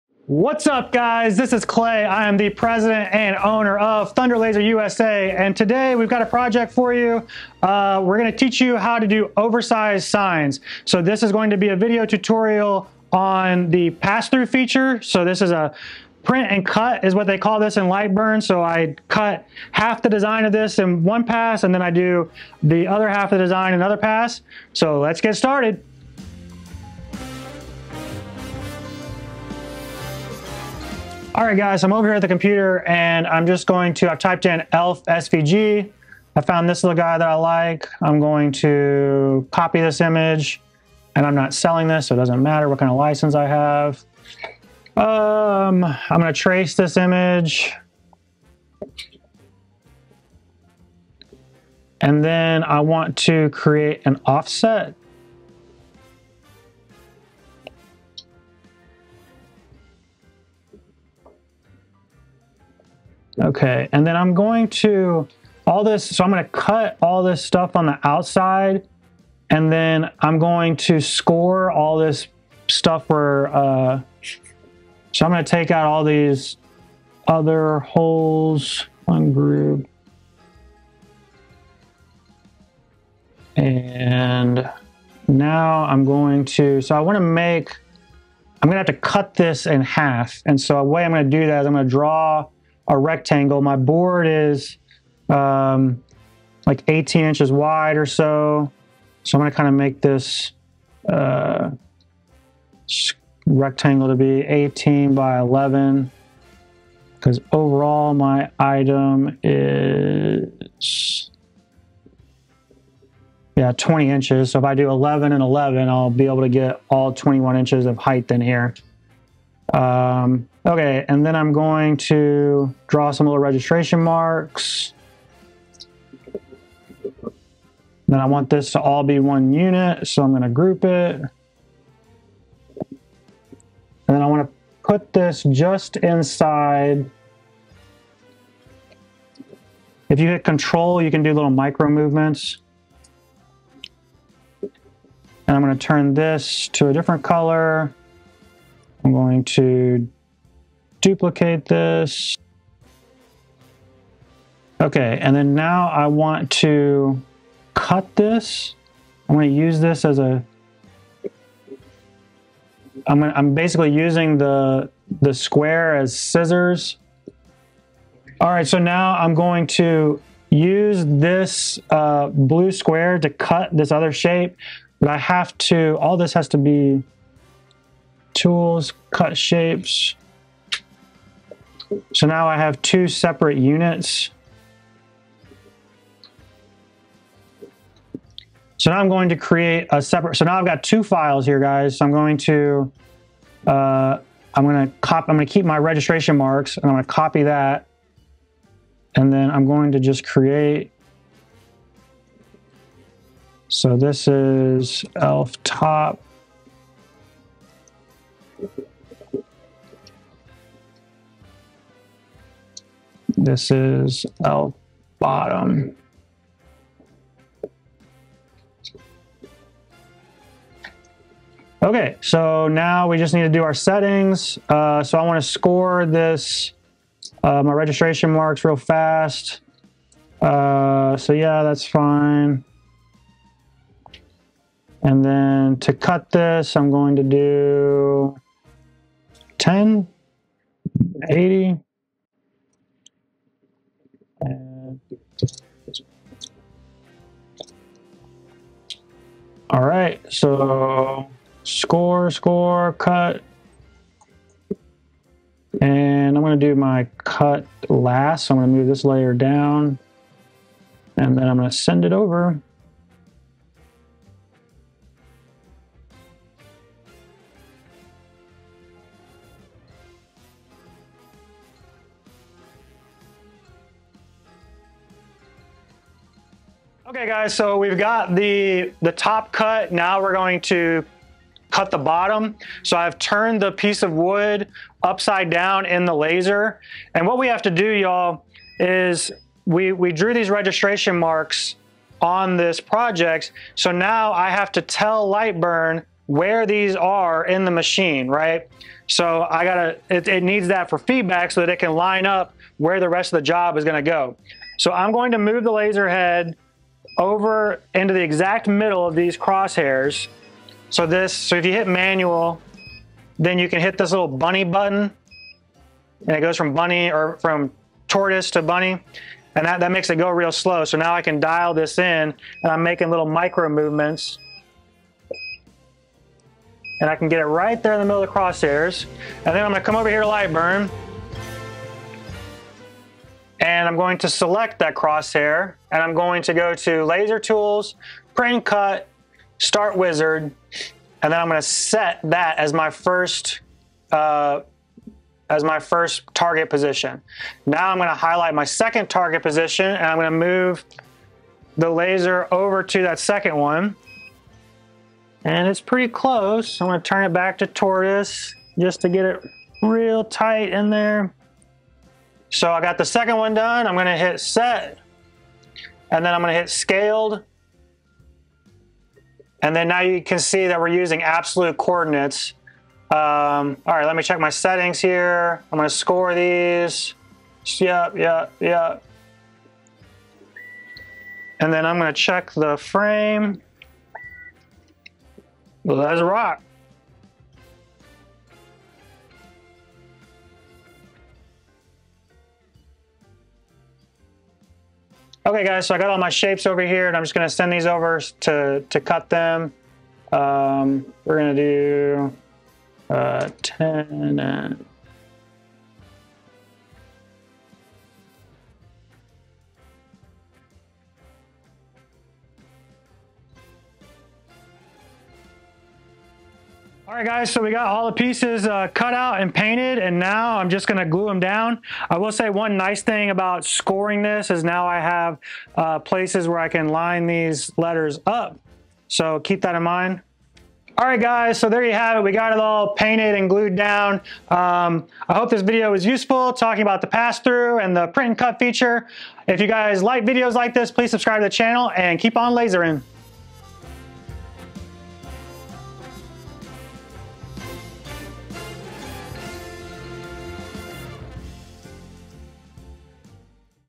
Clay's Beginner Series - Project 8 - Elf Sign (Passthrough)
Ready to level up your laser projects? In this tutorial, Clay, the President and Owner of Thunder Laser USA, demonstrates how to use the pass-through feature in Lightburn to create oversized signs. Perfect for holiday crafts, this step-by-step guide shows how to design, cut, and assemble a 20-inch tall elf sign.

📋 What You’ll Learn in This Video:

- How to find and trace an PNG file.
- Setting up designs with Lightburn's print-and-cut feature.
- Using the pass-through feature for large-scale projects.
- Tips for precise registration marks and alignment.
- Painting and assembling the final design.

✨ Whether you're new to laser cutting or a seasoned pro, this tutorial is packed with helpful tips to improve your workflow and expand your creative possibilities.

0:00 Intro
0:26 File Setup
4:23 Top Cut
4:35 Bottom Cut
6:32 Cut Letters & Shapes
6:55 Paint and Glue
7:11 Outro